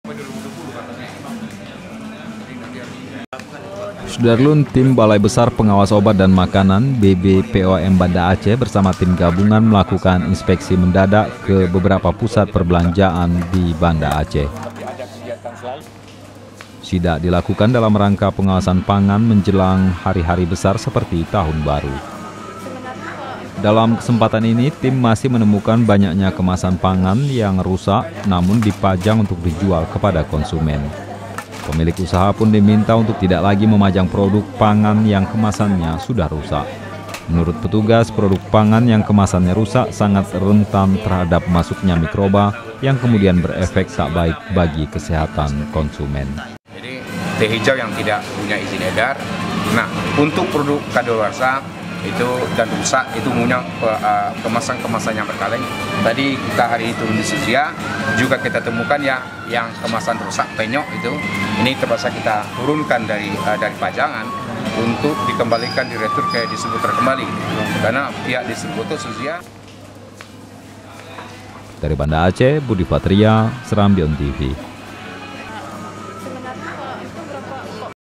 Hati Hati Berberlanja, BBPOM Banda Aceh Masih Temukan Produk Pangan Dengan Kemasan Rusak
SERAMBITV.COM, BANDA ACEH - Tim Balai Besar Pengawas Obat dan Makanan (BBPOM) Banda Aceh bersama tim gabungan melakukan inspeksi mendadak ke beberapa pusat perbelanjaan di Banda Aceh, Selasa (18/12/2018).

Sidak dilakukan dalam rangka pengawasan pangan menjelang hari-hari besar seperti tahun baru.

Dalam kesempatan ini tim masih menemukan banyaknya kemasan pangan yang rusak, namun dipajang untuk dijual kepada konsumen.

Pemilik usaha pun diminta untuk tidak lagi memajang produk pangan yang kemasannya sudah rusak.

Menurut petugas, produk pangan yang kemasannya rusak sangat rentan terhadap masuknya mikroba, yang kemudian berefek tak baik bagi kesehatan konsumen.